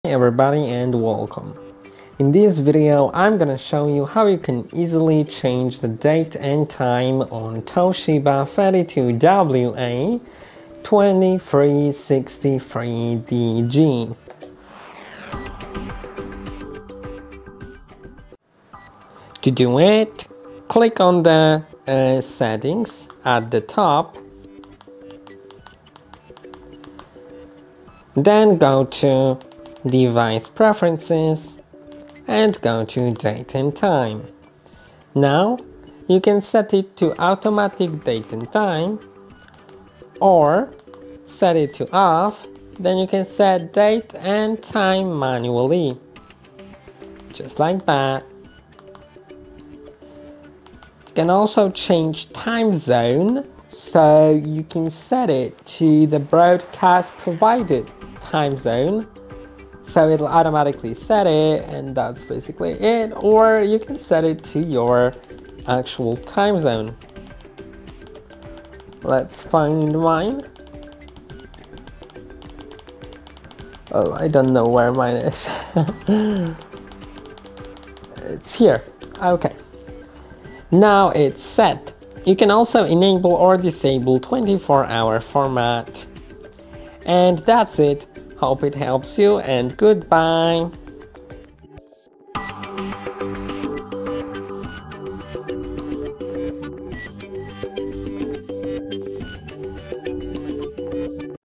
TOSHIBA Android TV – How to Change Date and Time
If you need to update the date and time settings on your TOSHIBA Android TV, this video will guide you through each step. Learn how to access the settings menu, adjust the date and time manually or automatically, change the time zone, and enable or disable the 24-hour format on your TOSHIBA 32WA2363DG or similar models. Keeping your TV’s date and time accurate is important for scheduled recordings, apps, and system updates. Follow along to make sure your TOSHIBA Android TV always shows the correct time and date.

How to change date and time on TOSHIBA Android TV?
How to set time zone on TOSHIBA Android TV?
How to enable or disable 24-hour format on TOSHIBA Android TV?

0:00 Introduction
0:14 Open Settings Menu
0:28 Go to Device Preferences
0:34 Access Date and Time Settings
0:46 Set Date and Time Automatically or Manually
0:59 Change Time Zone
1:21 Enable or Disable 24-Hour Format
1:26 Summary